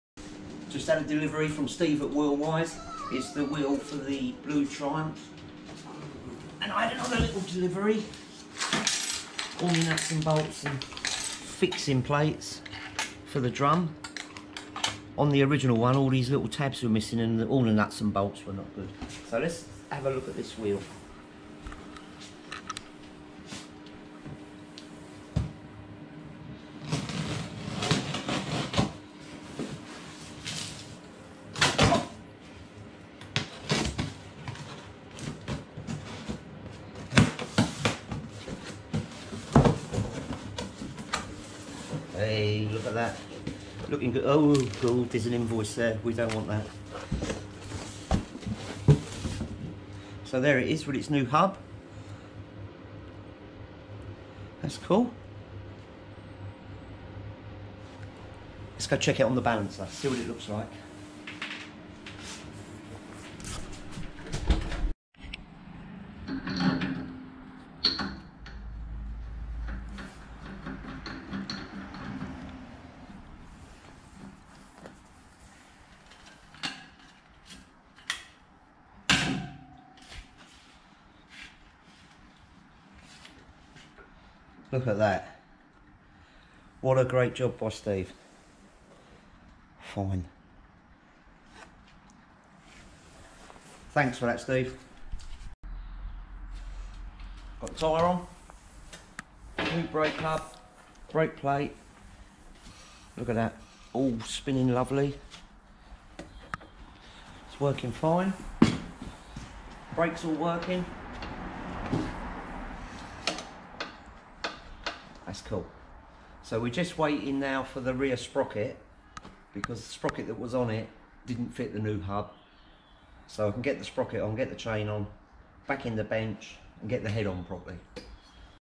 Really Wheely Good!
The rear wheel returns from the wheel builder (Wheelwise Engineering, Cowfold) and Tony is keen to get it fitted back in the bike.